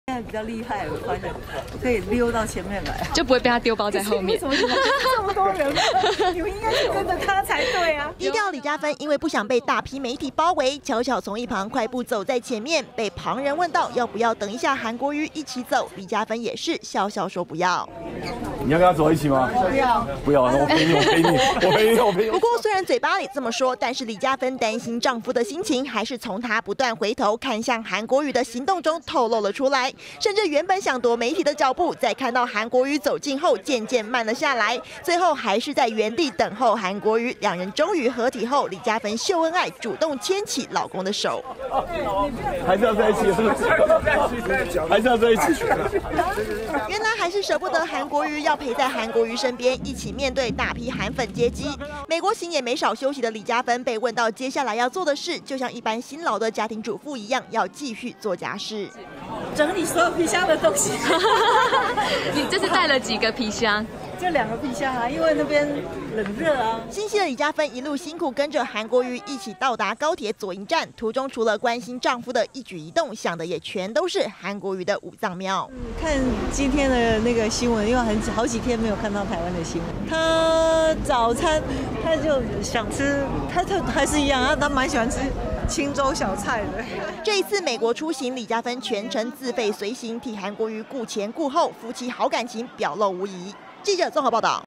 20190418中天新聞 李佳芬低調怕媒體包圍 想丟包韓又捨不得
韓國瑜訪美行終於結束，和李佳芬兩人18號，回到台灣一下飛機，低調的李佳芬，為了不想被大批媒體包圍，沒等韓國瑜就先行往前快步走，但是最後還是因為放心不下老公，又主動回頭，去牽韓國瑜的手，兩人這才再度合體 ，一起和韓粉見面，夫妻的好感情，再度讓人羨慕。